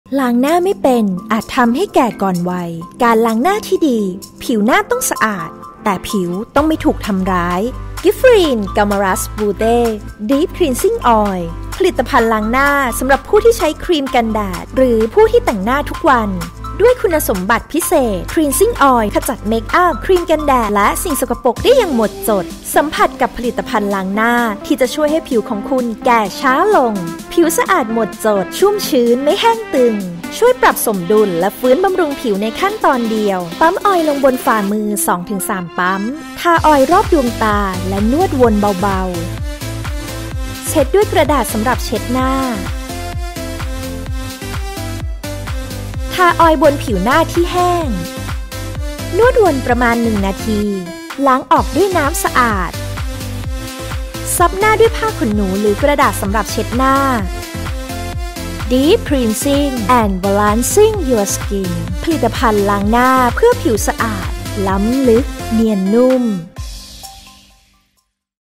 กิฟฟารีน กลามอรัส บูเต้ ดีพ คลีนซิ่ง ออยล์
ล้างหน้าไม่เป็นอาจทำให้แก่ก่อนวัย...

การล้างหน้าที่ดีเป็นอย่างไร...ไปชมกันค่ะ